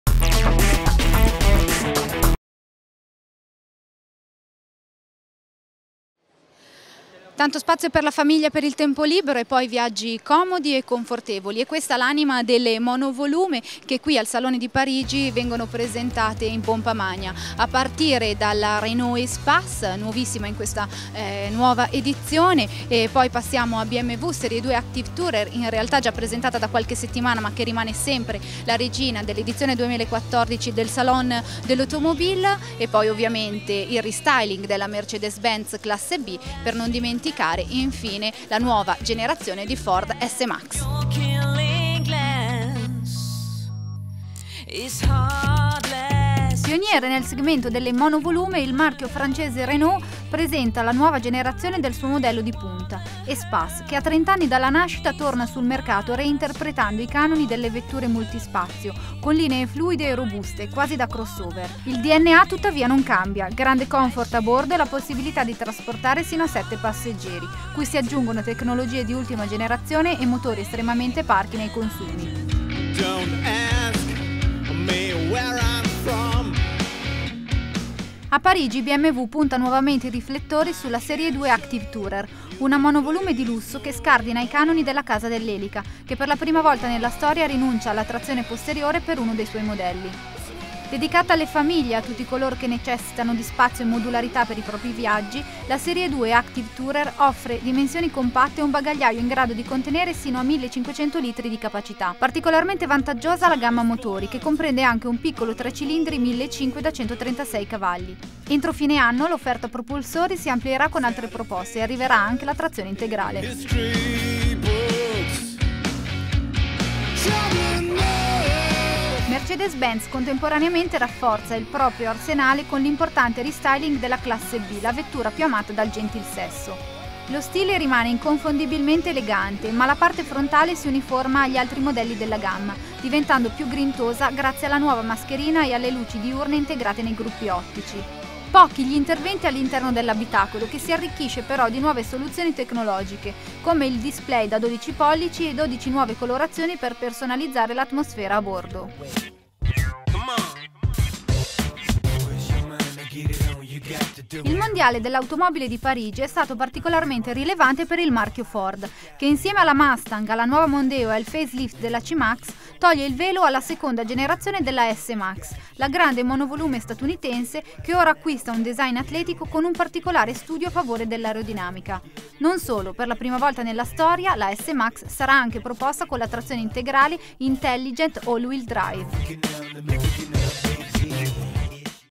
Tutte le Monovolume esposte al salone dell'auto di Parigi 2014!
La pillola di questa settimana ci porta dritti dritti all'interno del Salone dell'Automobile di Parigi. In questa clip troverete tutte le novità riguardanti le monovolume. Non perdetevele.

Se ti è piaciuto questo video non perderti quello riguardante le nuove supercar esposte in anteprima al Salone di Parigi.

aggiornato su tutte le novità dal mondo dei motori.